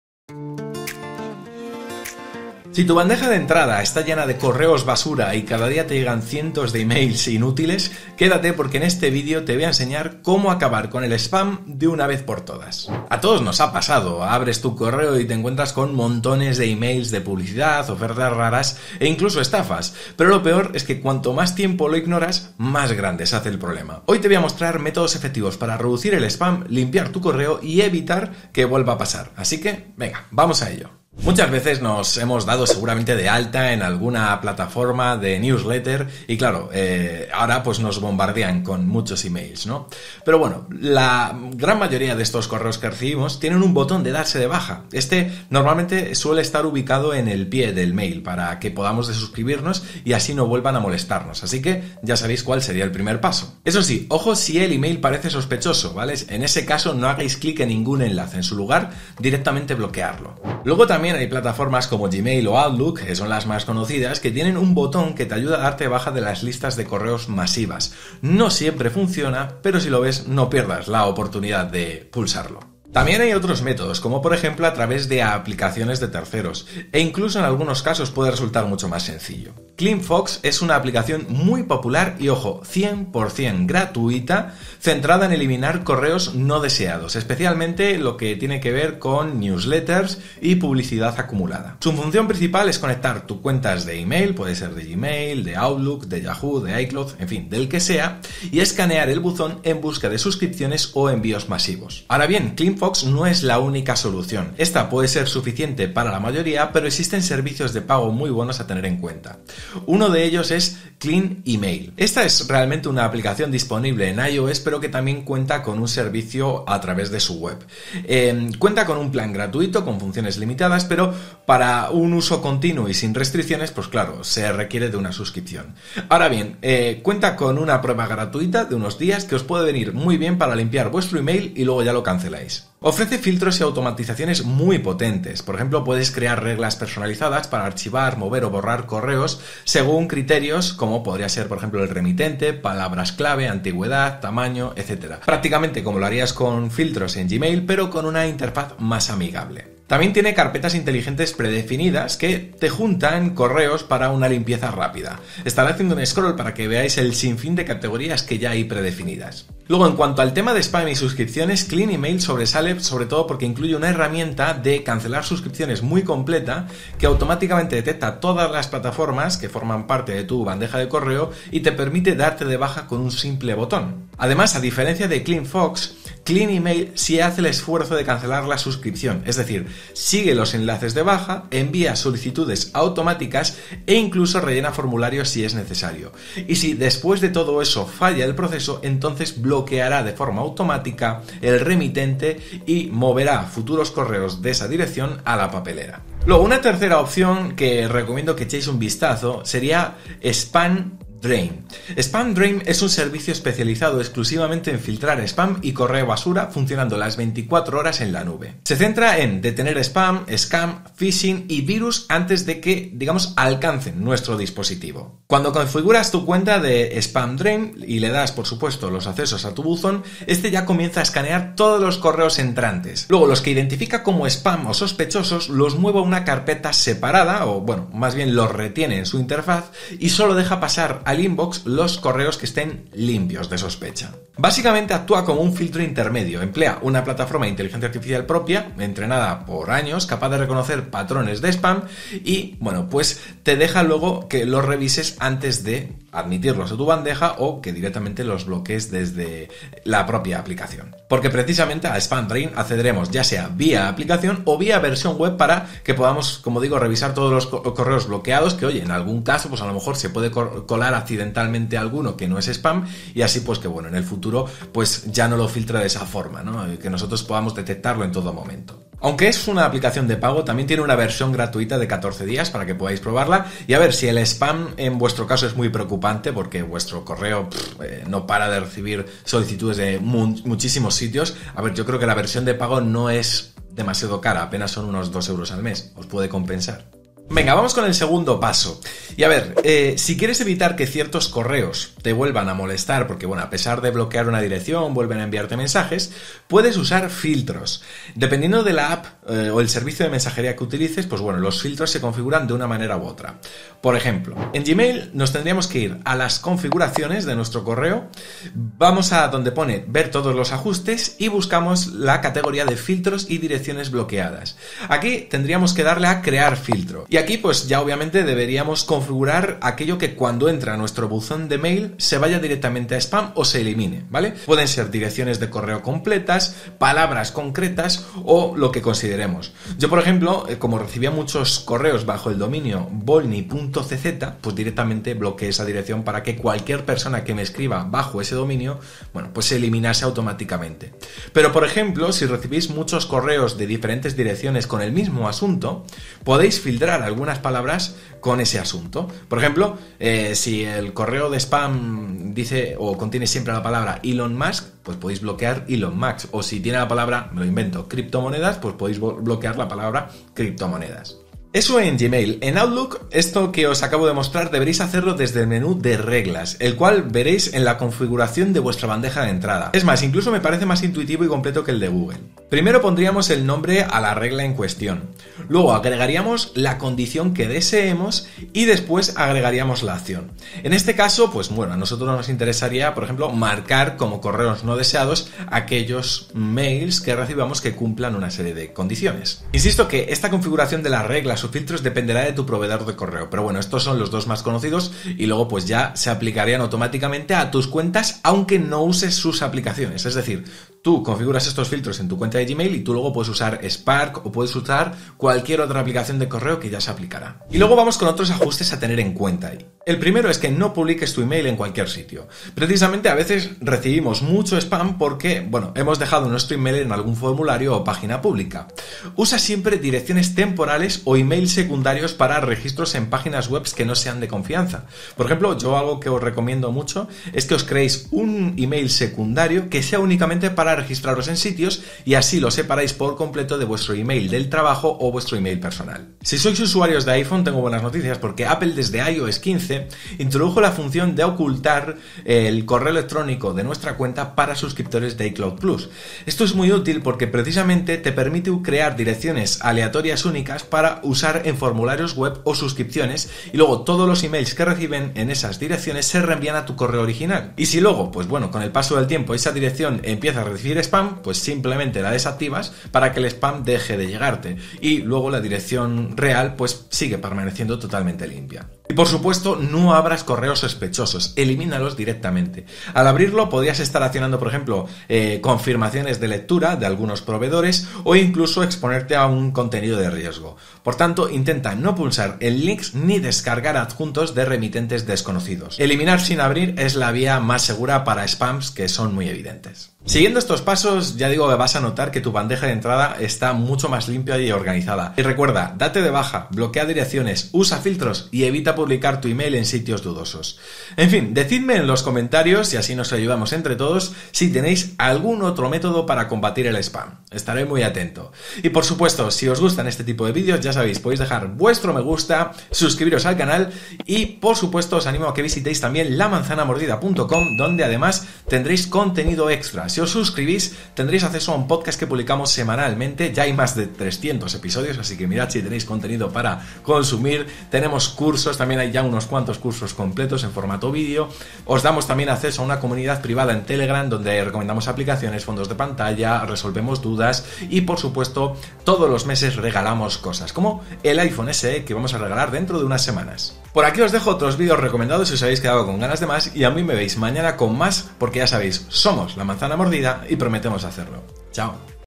Cómo ELIMINAR correos basura y evitar el SPAM en Gmail y Outlook
El spam puede llenar tu bandeja de entrada de correos basura, pero hay soluciones efectivas. En este video, te enseño cómo eliminar correos no deseados en Gmail y Outlook, filtrar spam y evitar que siga llegando. Aprenderás trucos como darse de baja de suscripciones, usar filtros automáticos, y descubrir las mejores apps para limpiar tu correo. Si buscas cómo eliminar correos basura de Gmail o Outlook, este video es para ti.

🎬 Momentos destacados del vídeo:
0:00 Introducción: Cómo acabar con el spam en tu bandeja
0:31 Cancelar suscripciones: Baja de newsletters y bloquea correos sospechosos
1:13 CleanFox: Elimina newsletters y publicidad no deseada gratis
1:49 Clean Email: Automatiza cancelación de suscripciones y organiza tu correo
3:16 SpamDrain: Filtro en la nube contra spam, scam y phishing
4:47 Configura filtros en Gmail y Outlook para bloquear correos no deseados
6:47 Protege tu email: Consejos para evitar spam y mantener tu bandeja limpia
8:20 Conclusión y CTA: Optimiza tu correo y descubre más tips

✨ Etiquetas del video